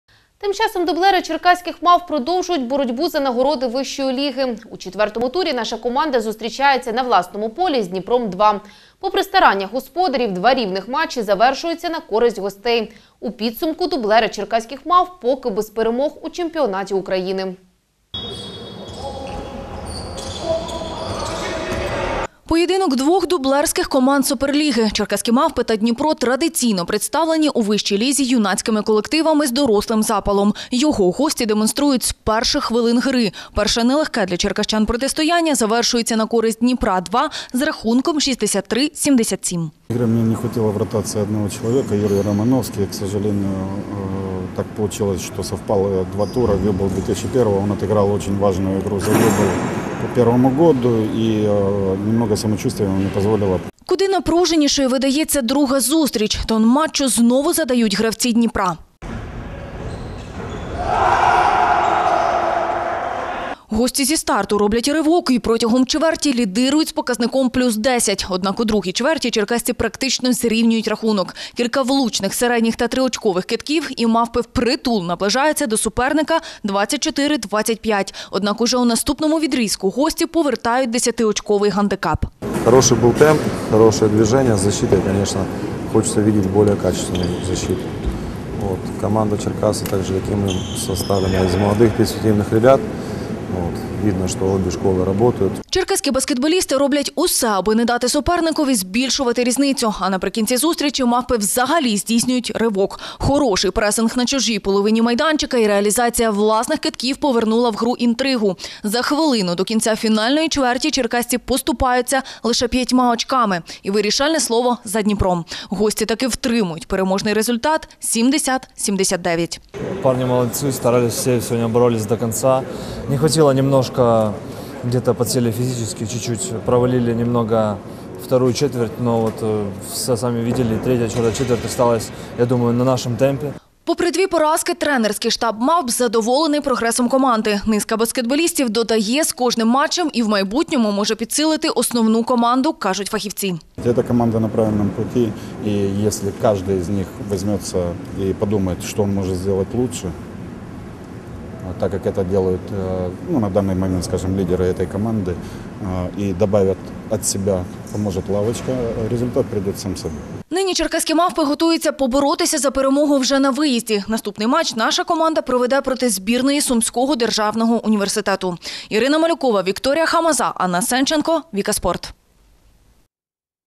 «Черкаські Мавпи-2» поступилися дублерам «Дніпра» в боротьбі за нагороди Вищої ліги
У четвертому турі наша команда зустрічається на власному полі з «Дніпром-2»